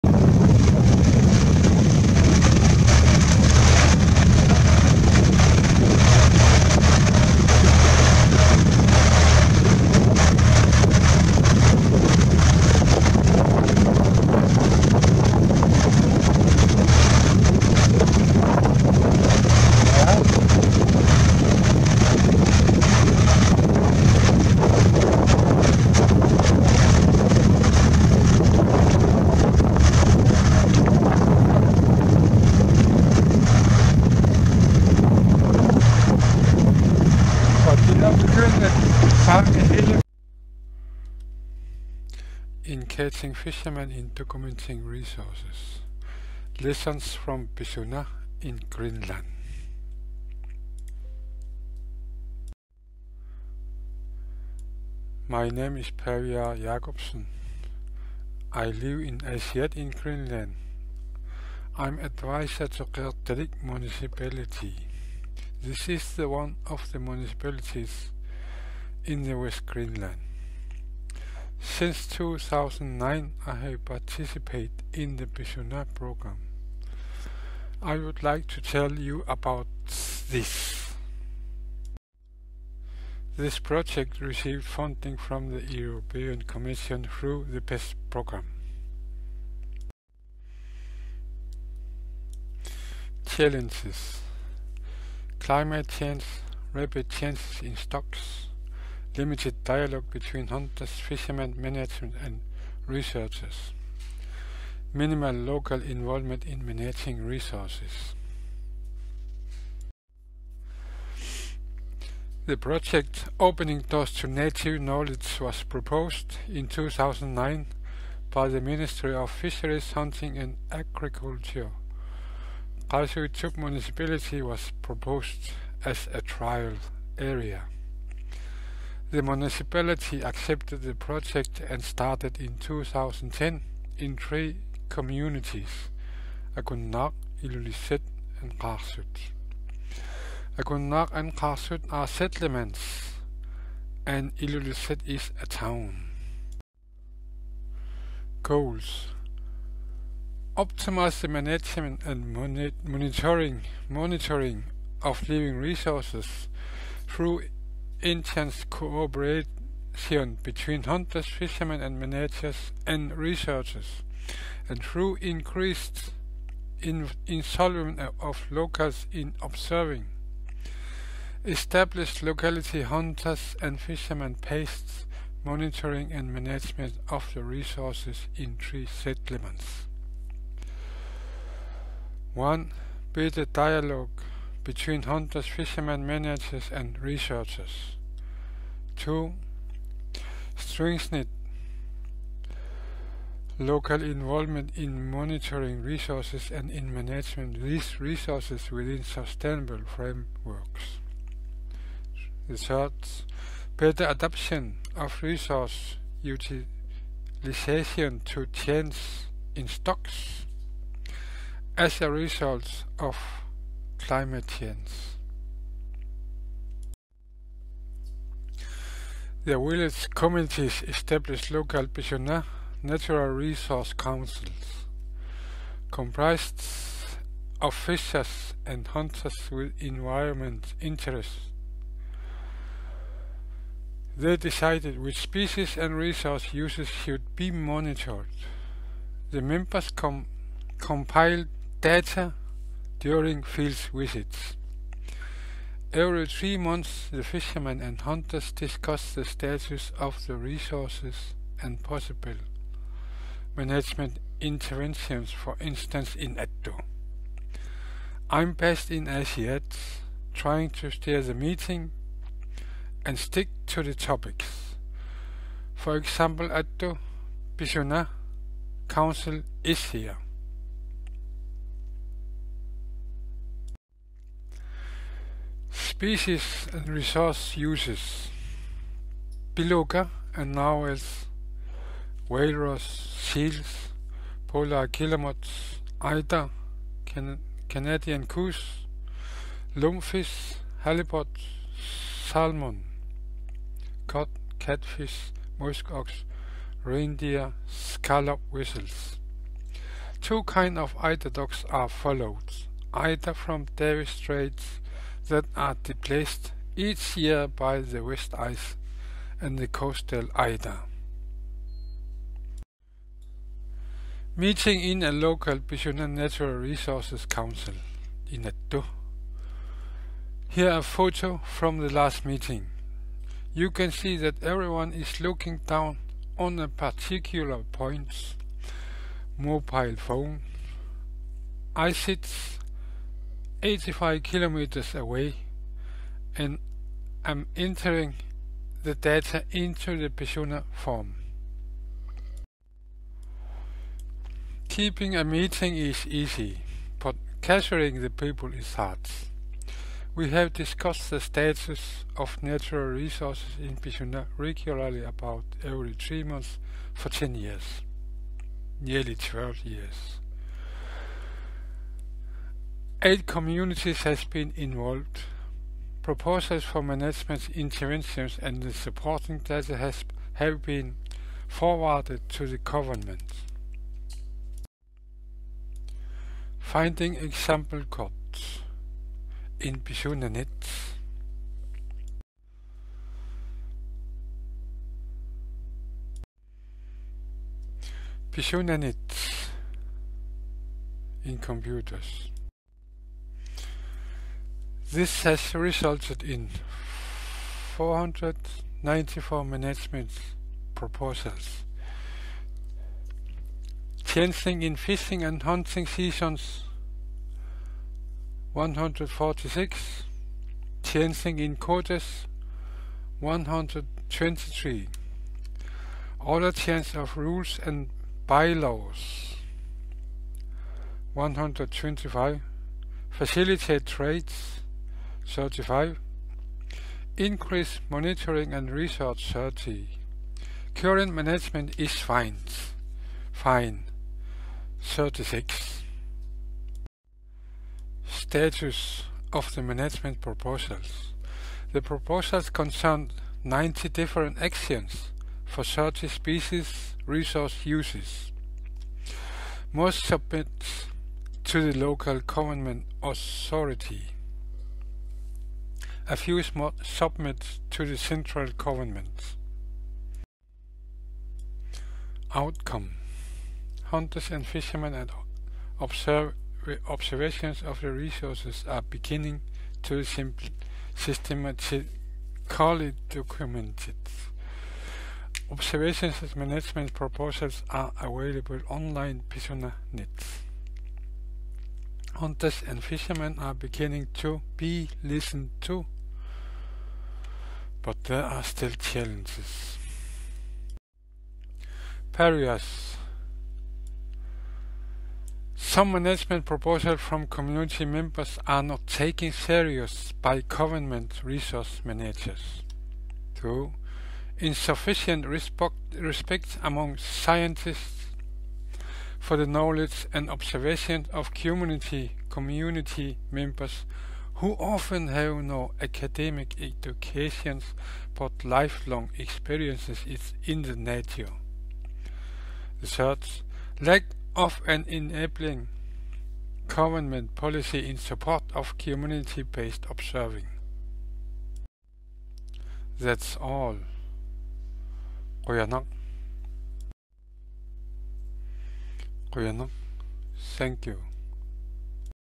Engaging fishermen in documenting resources: Lessons from PISUNA in Greenland. PâviâraK Jakobsen.
Engaging fishermen in documenting resources: Lessons from PISUNA in Greenland. PâviâraK Jakobsen, Qeqertalik Municipality, Greenland, 2021.
PISUNA Greenland presentation, World Conservation Congress, Marseille 2021